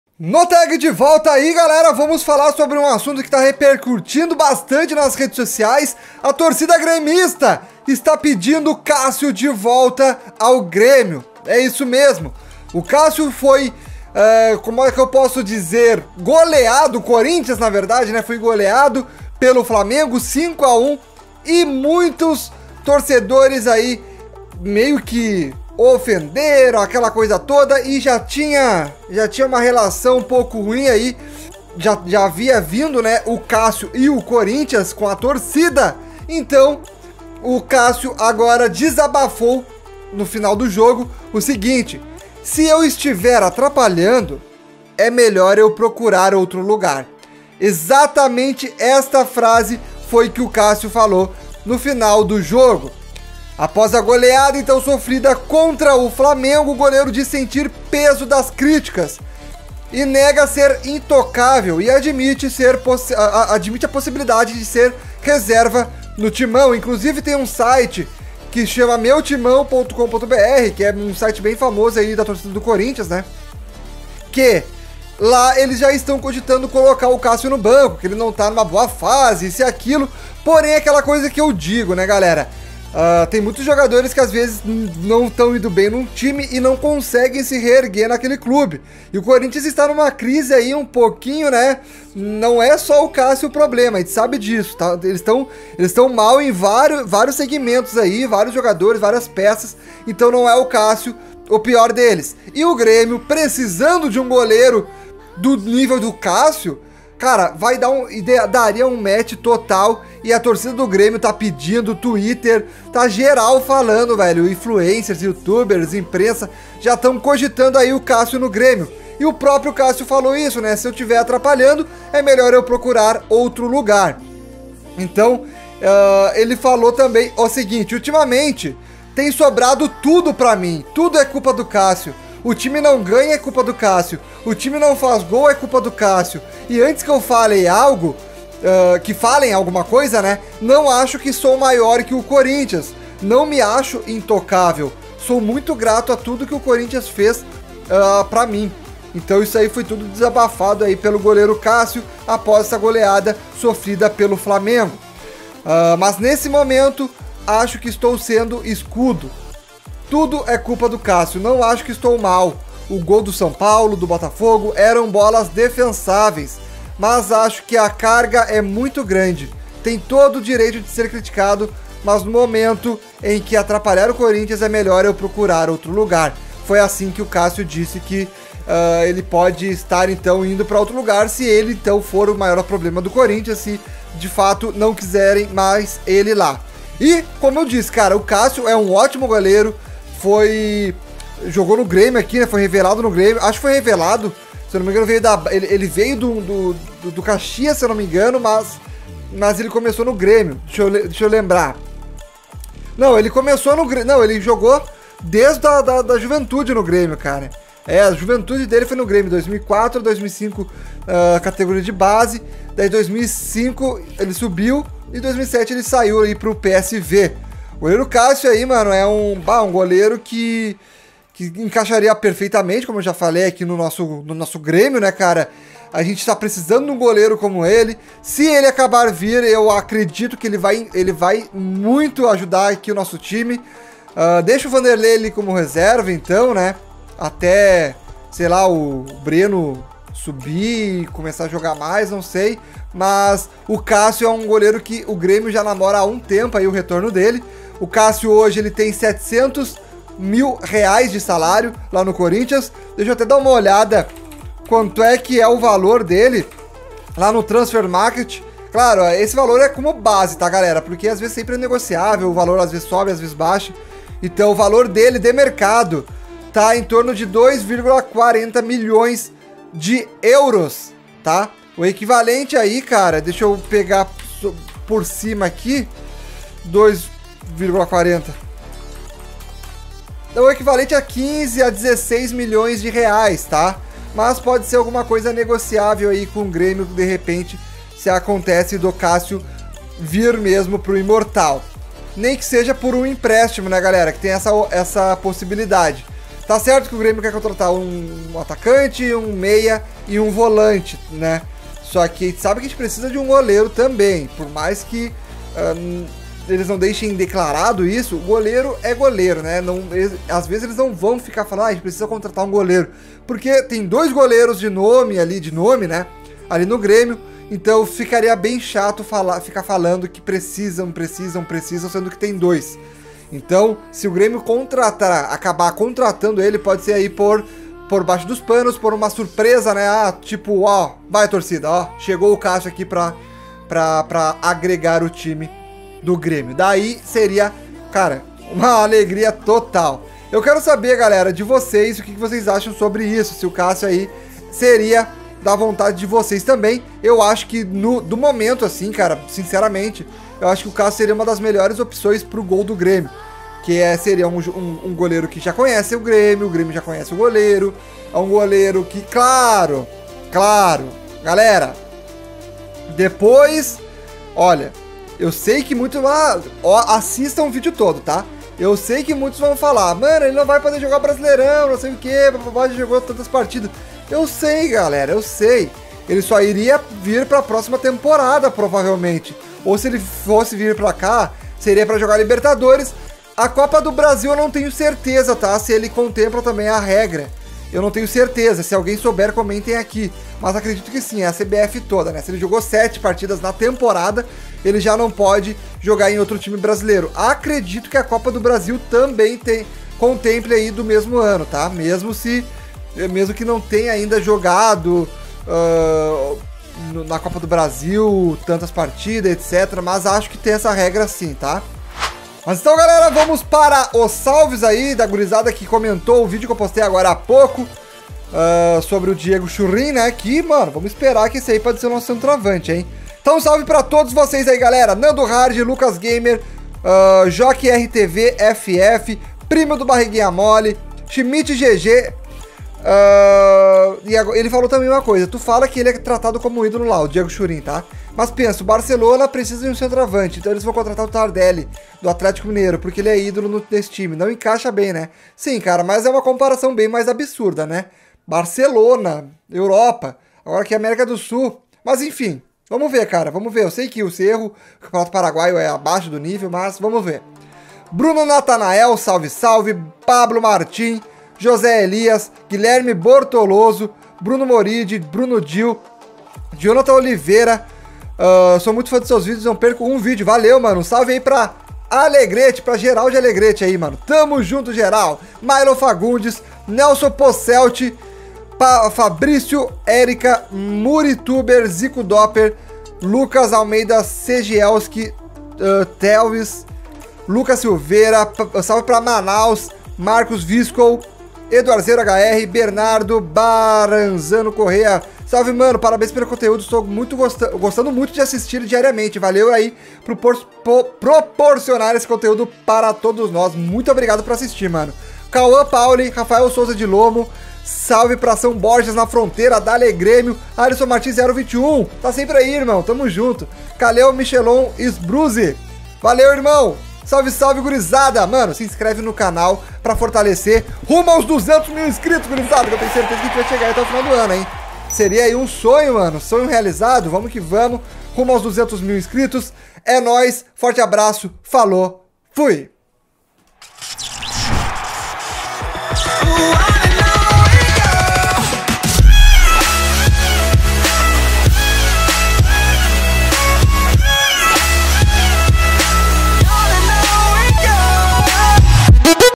CÁSSIO NO GRÊMIO? APÓS GOLEADA POR 5 a 1 PRO FLAMENGO CÁSSIO DESABAFA: "MELHOR PROCURAR OUTRO LUGAR"
Cássio sofre muitas críticas após goleada contra o Flamengo e a torcida do Grêmio pede seu retorno!